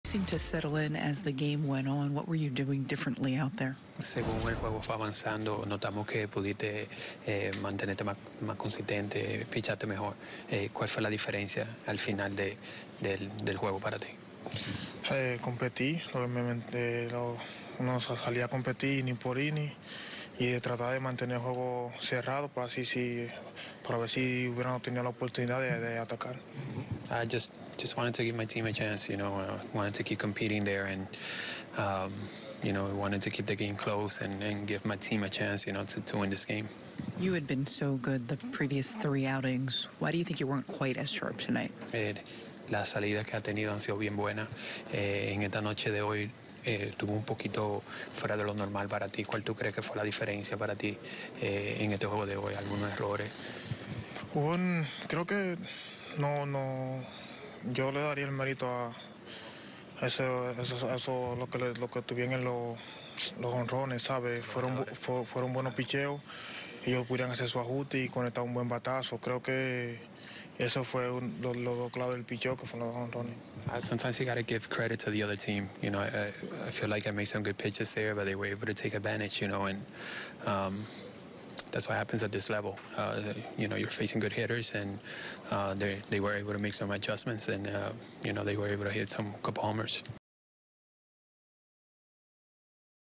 Domingo German strikes out 9 in quality start
For the 3rd time in his career as a starter, Domingo German struck out 9 and walked 0 batters despite the Yankees' loss to the Royals.